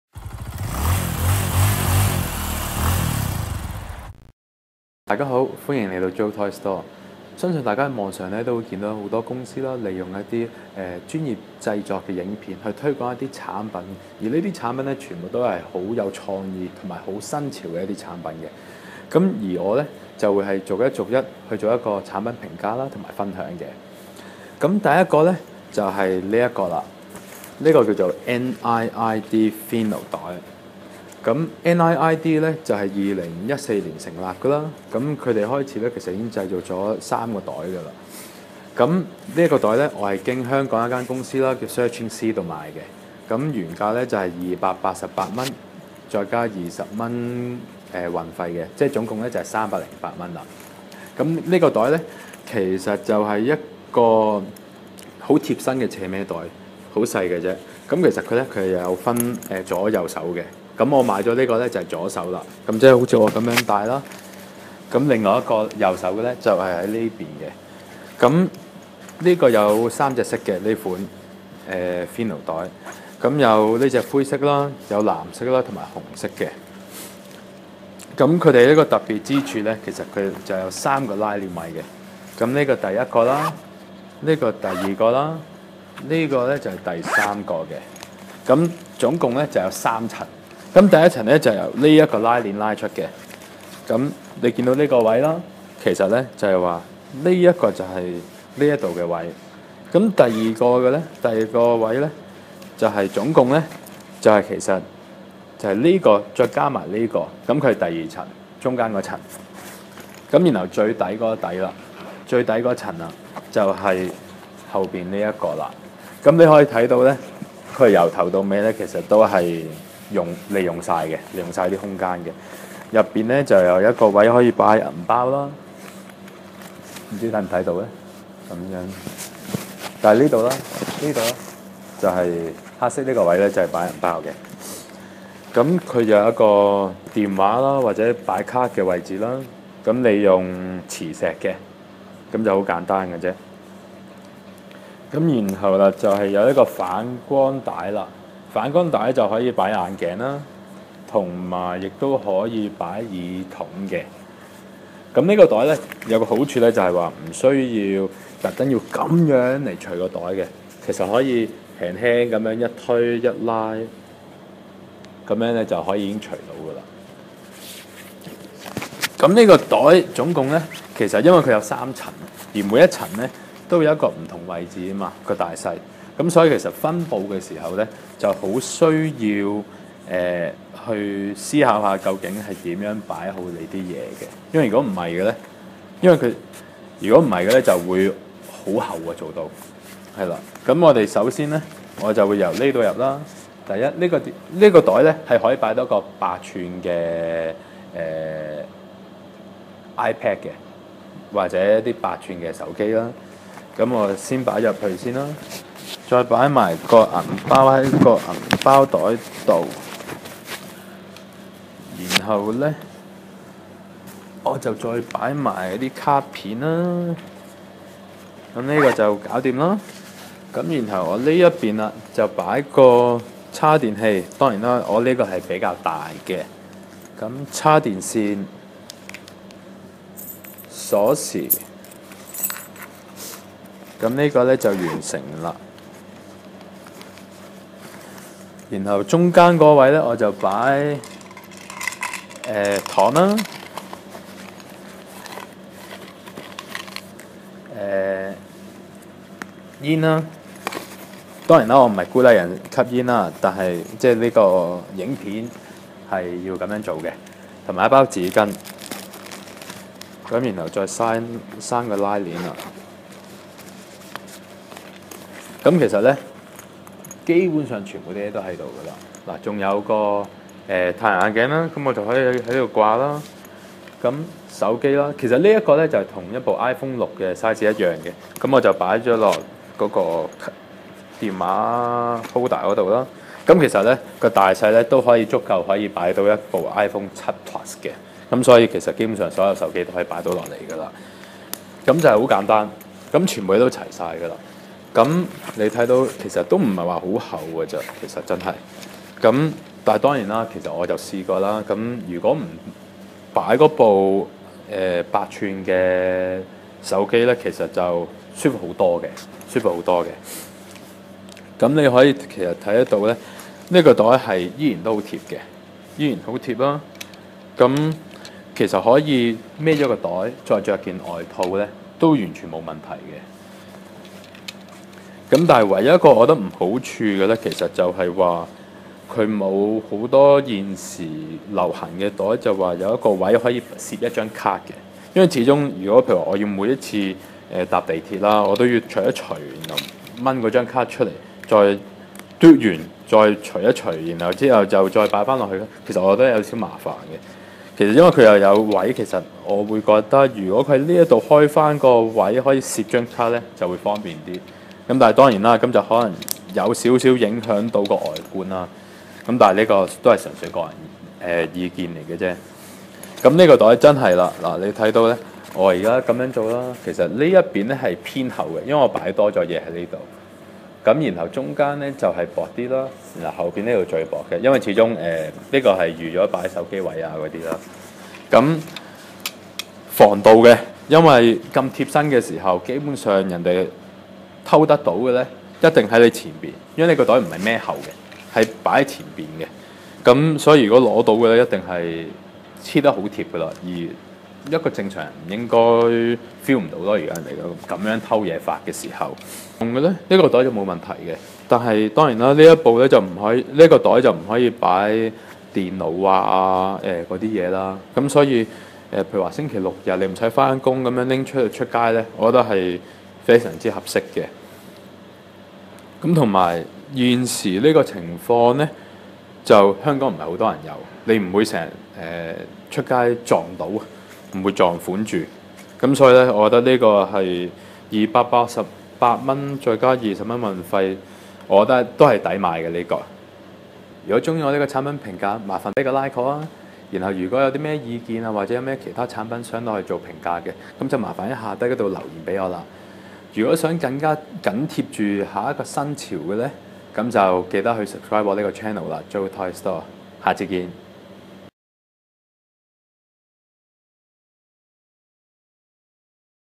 NIID FINO bag 評論 (中文版)
日常用超好!
外貌: 8, 非常獨特
舒適: 7 有8" tablet / 10 沒有8" tablet
實用性: 9, 如果FINO有一個專用的卡holder的話, 會是10